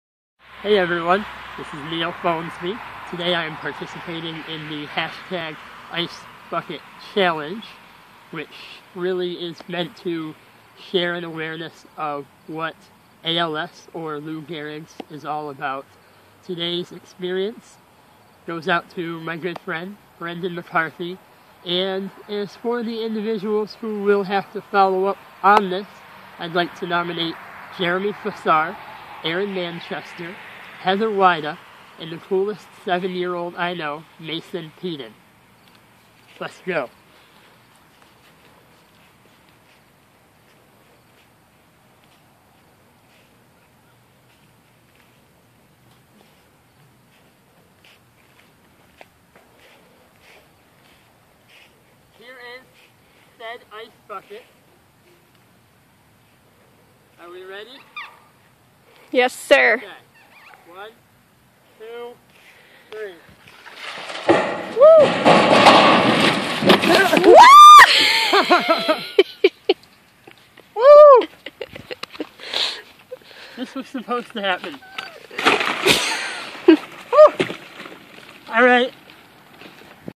Funny ALS Ice Bucket Challenge Flop
A unique twist on the ALS icebucketchallenge, with surprise ending! For each view, please donate $5 to my friend Brendan McCarthy, who is living with ALS, at